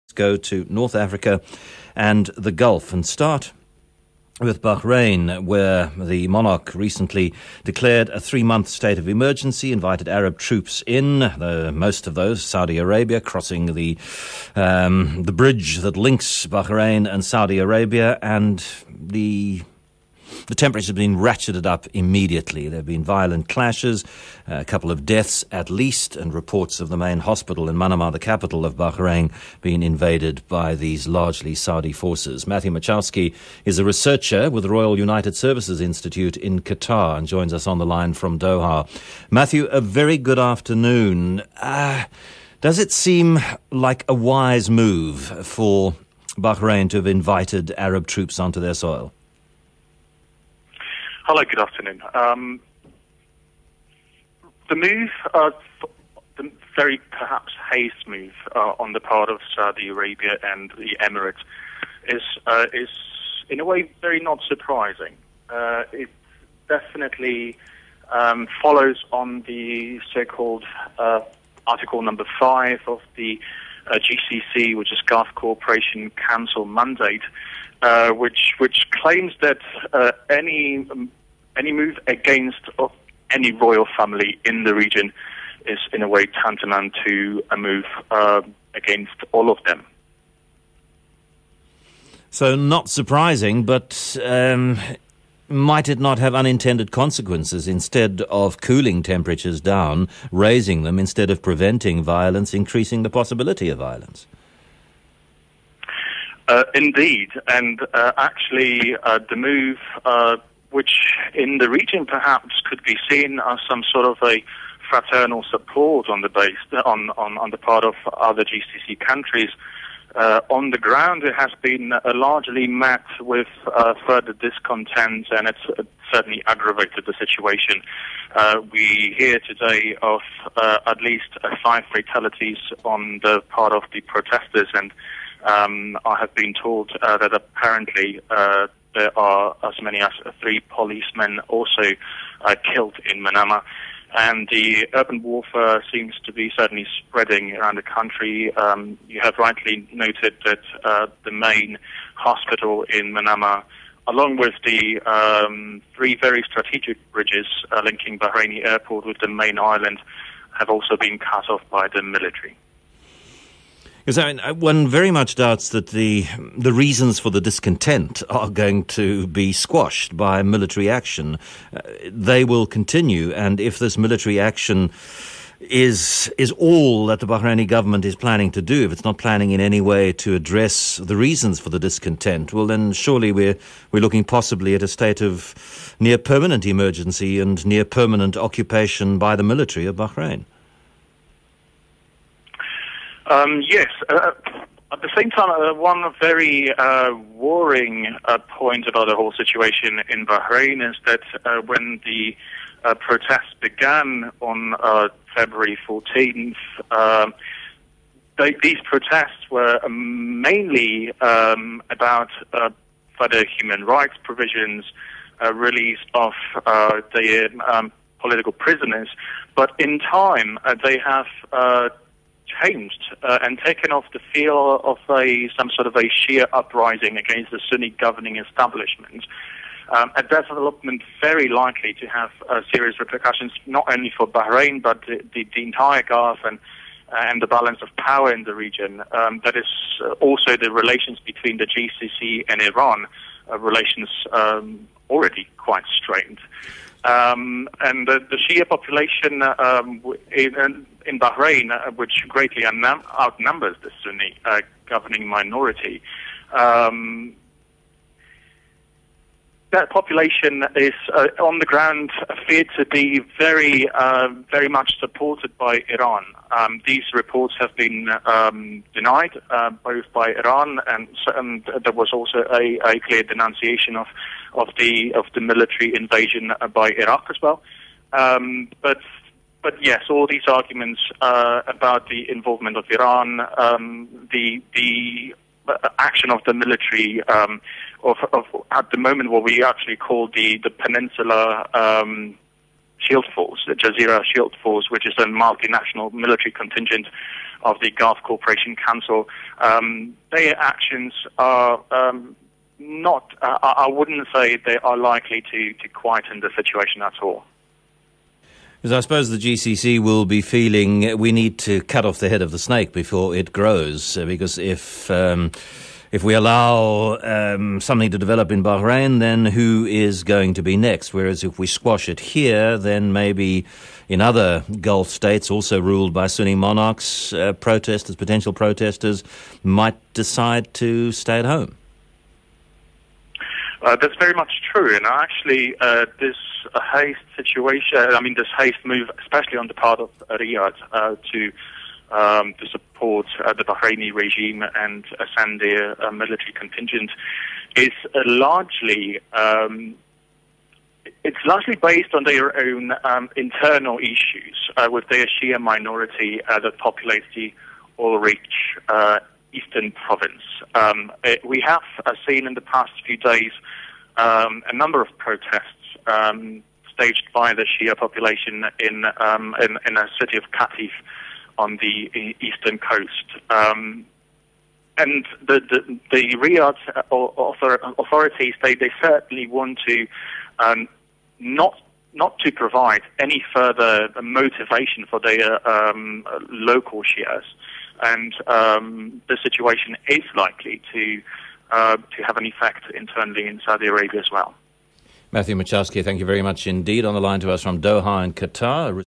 CapeTalk Radio: The Arab Spring arrives in Bahrain
In this interview I discuss the recent Saudi intervention in Bahrain. Following unrest in Manama Riyadh and Abu Dhabi decided to employ the rarely used Al Jazeera Shield Force to support the Sunni-minority monarchy in Bahrain. This intervention has since been seen as effective invasion of the country that led to numerous human rights abuses and killings.